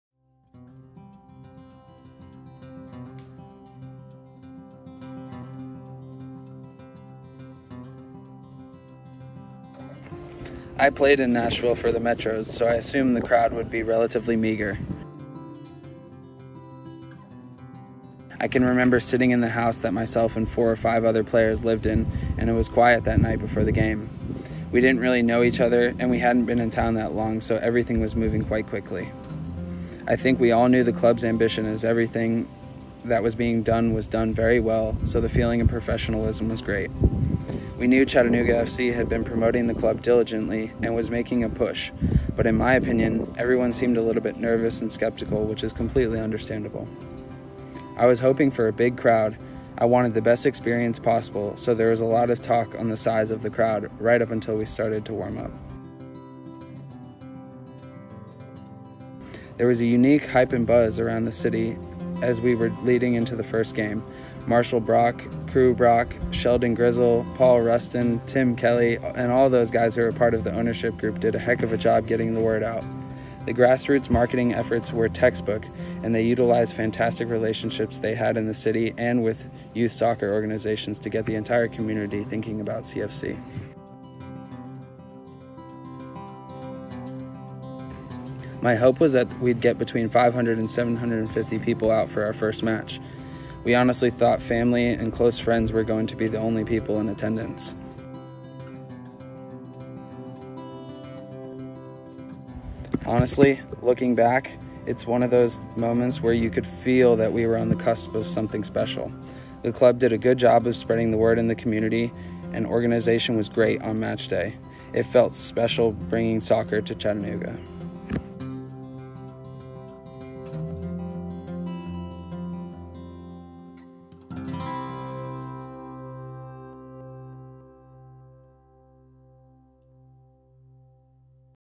Founder's Day - The Night Before
Players from the Inaugural First Team share their memories of the night before the first match